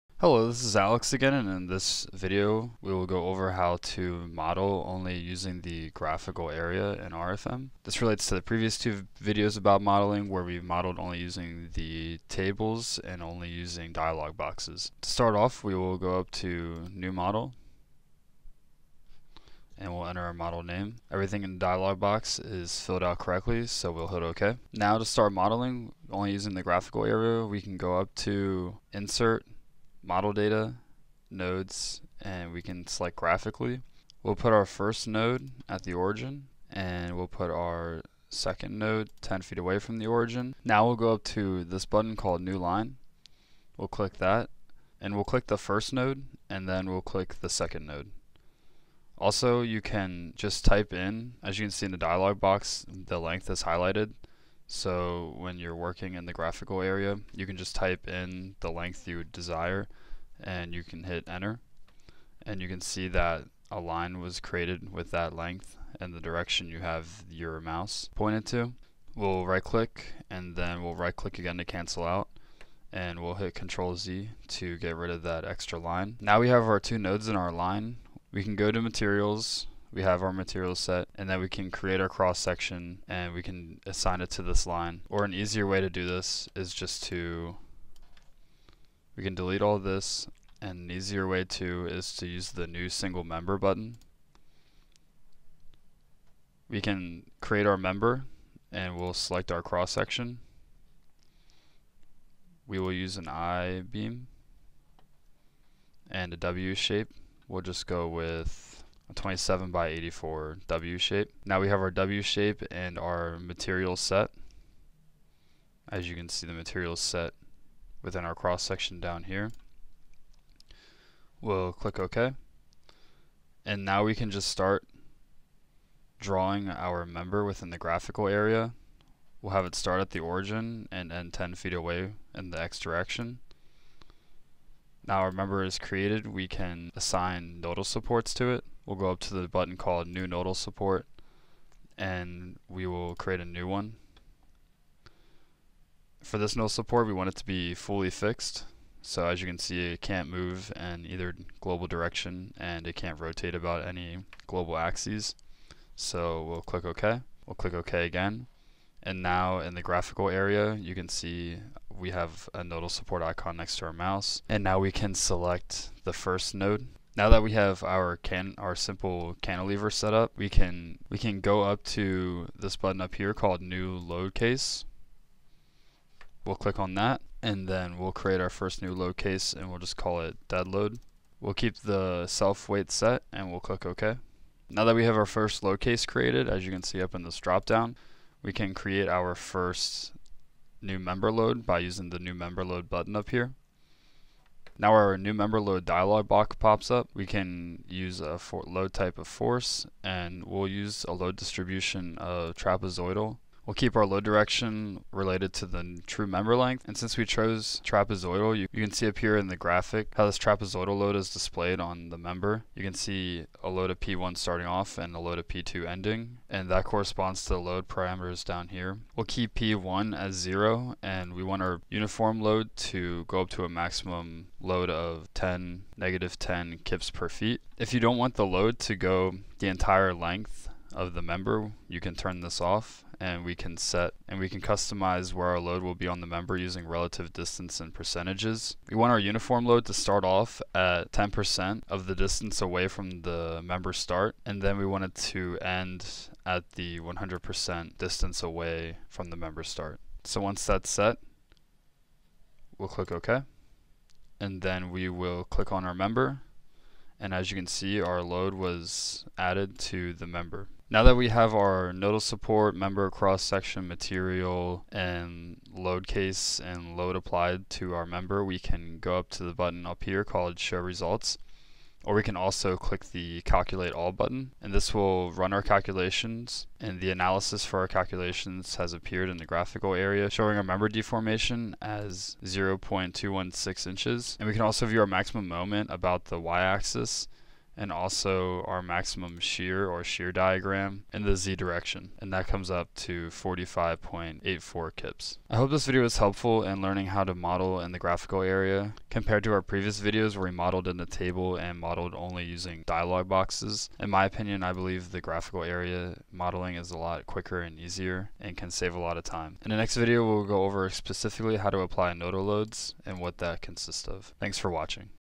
[EN] RFEM 5 Tutorial per studenti | 004 Modellazione mediante l'area grafica
Questo quarto breve video tutorial mostra come modellare in RFEM solo con l'area grafica. Il modello diventa una trave semplicemente supportata e i risultati vengono calcolati e analizzati.

Hai trovato utile questo video?

Approfitta della nostra assistenza gratuita. Ha altre domande?